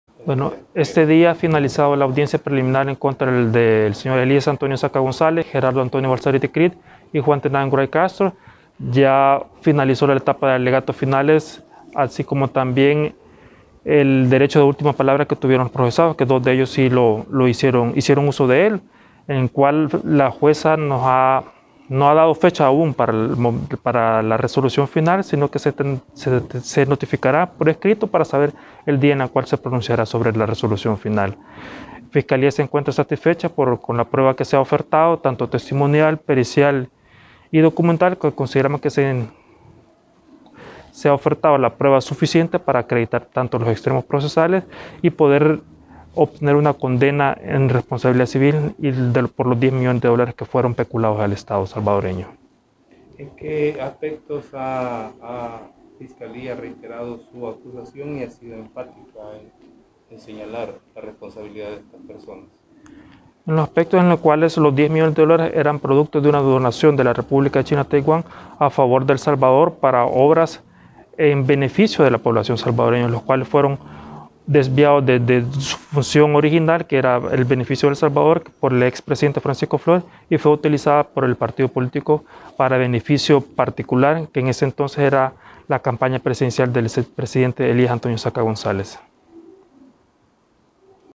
CasoChinaTaiwán I Finaliza la Audiencia Preliminar contra Elías Antonio Saca, Gerardo Antonio...
CasoChinaTaiwán I Finaliza la Audiencia Preliminar contra Elías Antonio Saca, Gerardo Antonio Balzaretti, Juan Tennant Wright acusados de haber desvíado $10 millones de dólares hacia las cuentas de ARENA.